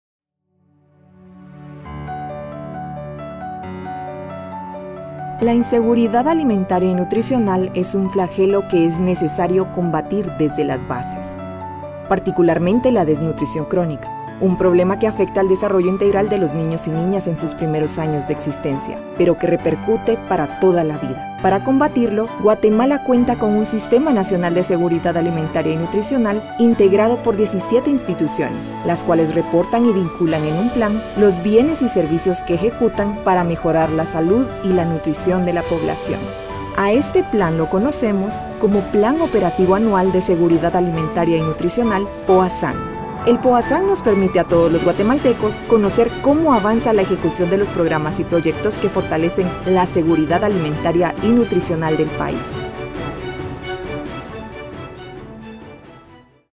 #1 Plan Operativo Anual de Seguridad Alimentaria y Nutricional
🔘El Plan Operativo Anual de Seguridad Alimentaria y Nutricional (POASAN) es una herramienta que permite programar y monitorear las intervenciones del Gobierno de Guatemala para fortalecer la Seguridad Alimentaria y Nutricional del país